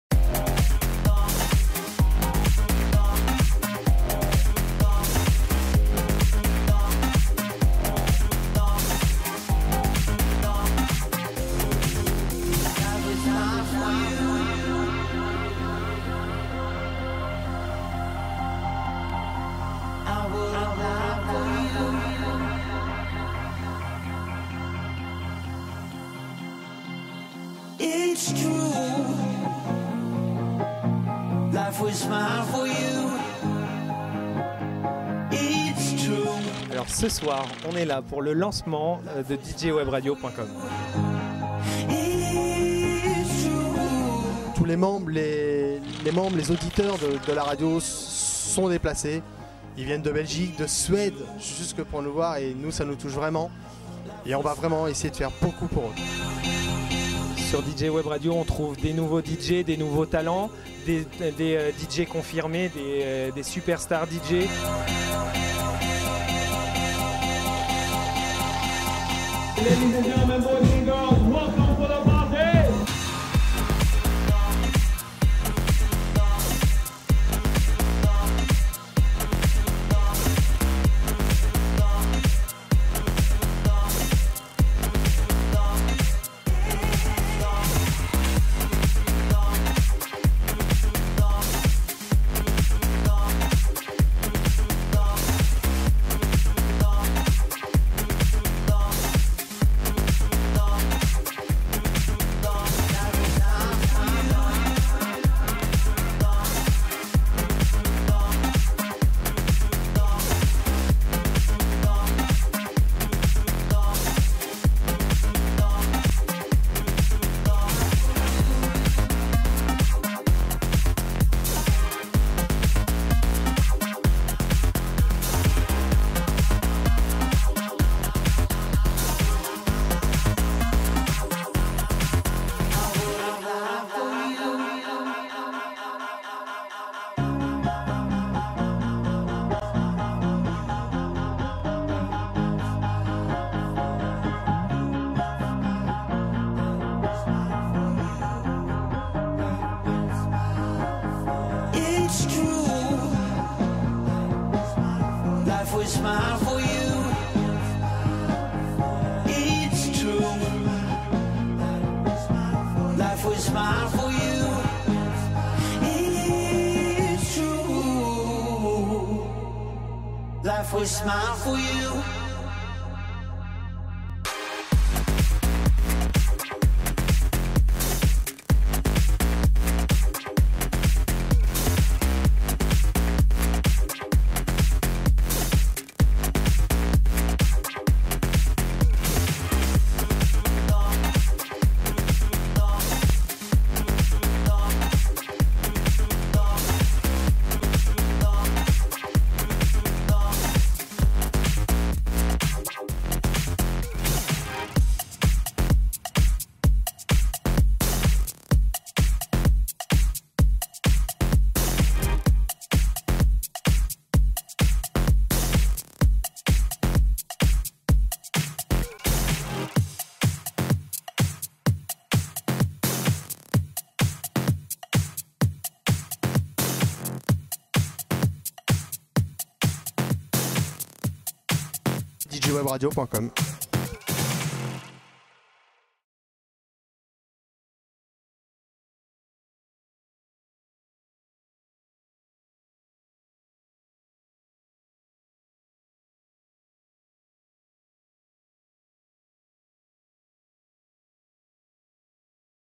Soirée d'inauguration DjWebRadio.com
Le 10 avril 2008, à Paris au ciné aqua, c'était le feu. Plus de 571 personnes ont fêté avec nous l'ouverture de la nouvelle webradio, avec aux platines : Benjamin Franklin, MC Adrian, Greg Modi IN, Jed Wong, Romain Pelletti, Sébastien Dupuis, Lorenzo di gracio...